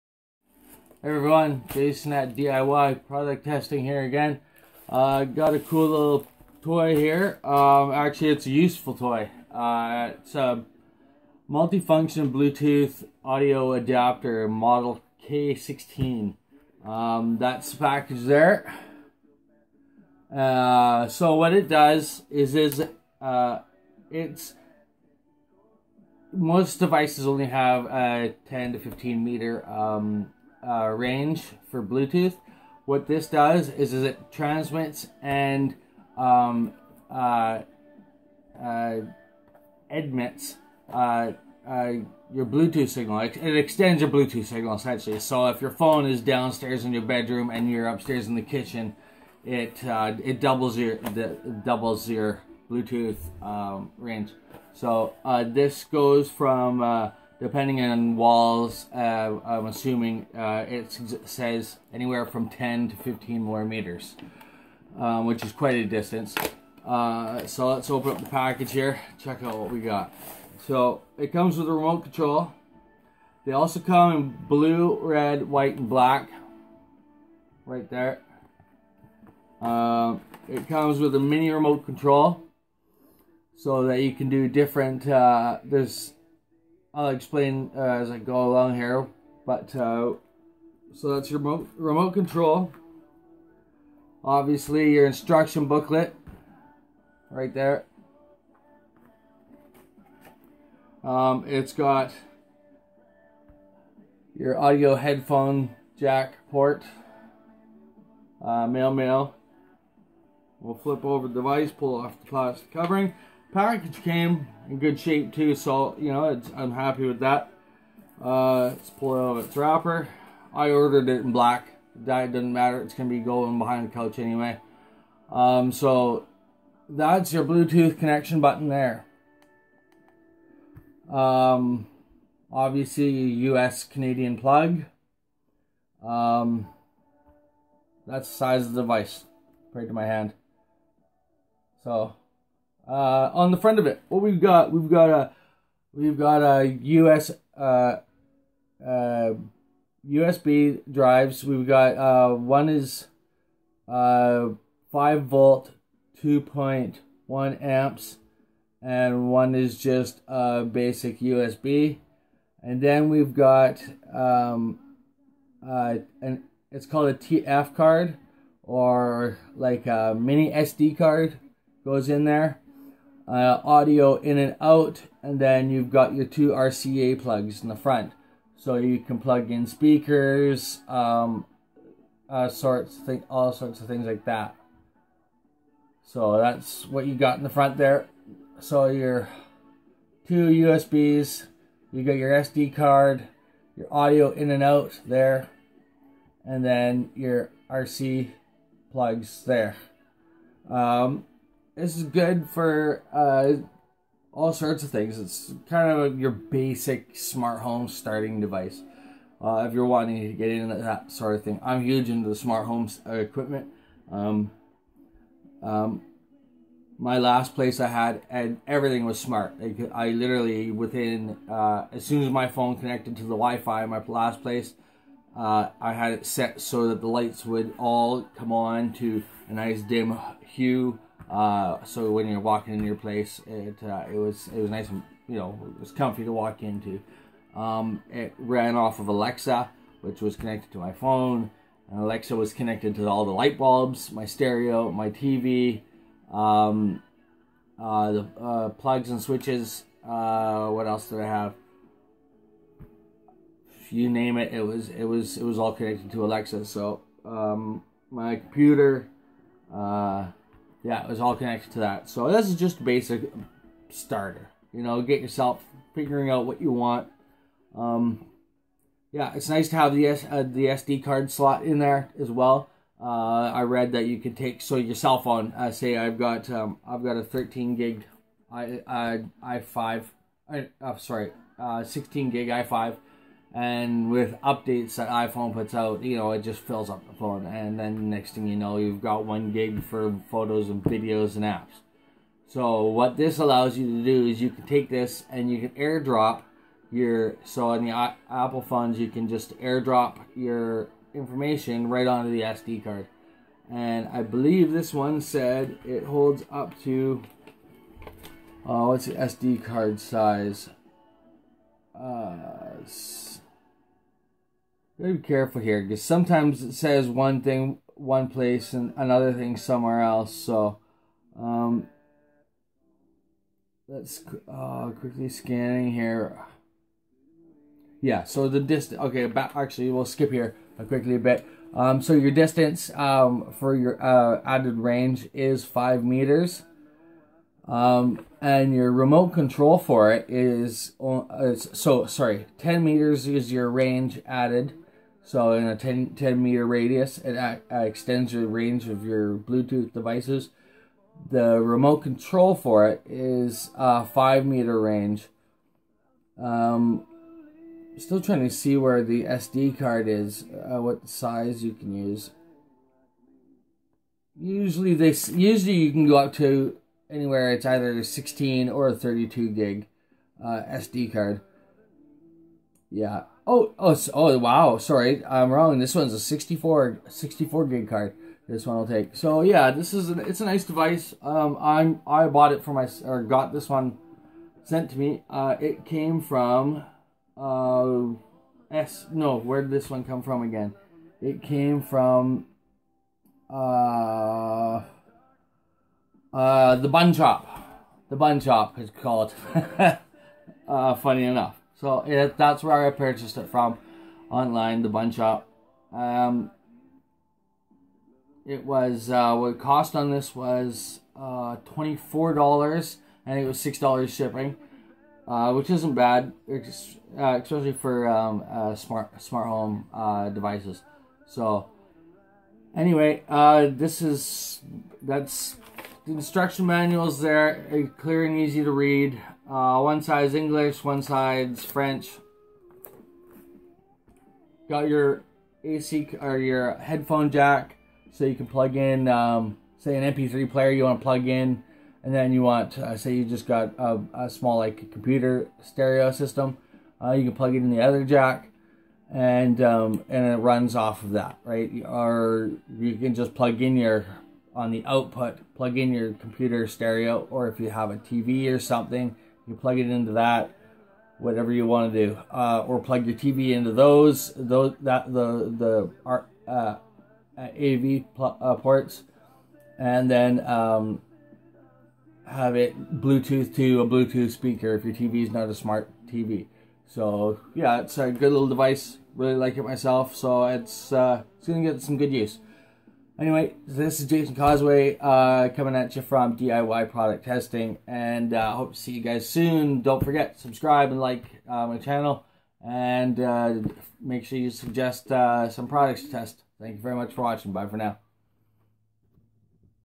multi-function Bluetooth audio adapter
this is the multifunction Bluetooth audio adapter model k-16 it's Bluetooth 5.0 range extender up to 10 metre range extender it helps receive and transmit Bluetooth signals in your house it can also be used as a personal cloud device for your household it takes a 36 gigabyte mini SD card it also has 2 RCA ports it has an in-and-out headphone jack lightning speed charger USB and a USB to plug in an external hard drive